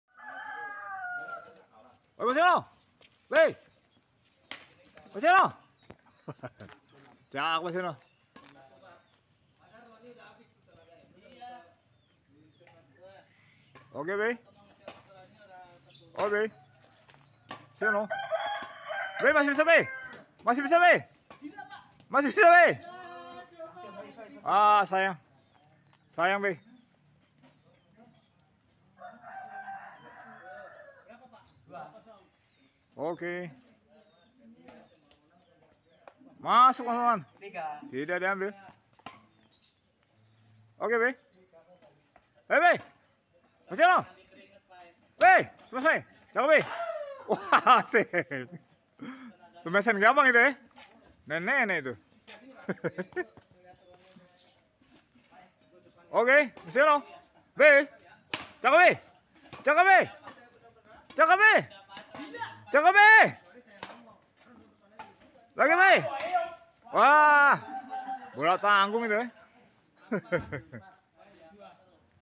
Aksi aksi Babe Cs. ada Smash an Si Pinter yg berhasil dan gagal.
Konten Ekyn Suka suka dr Channel : Eko Yuniswa @ 7172 dengan Tema Aksi aksi Babe Cs. ada Smash an Si Pinter yang berhasil dan gagal.

# Aksi aksi Babe Cs. ada Smash an Si Pinter yang berhasil dan gagal.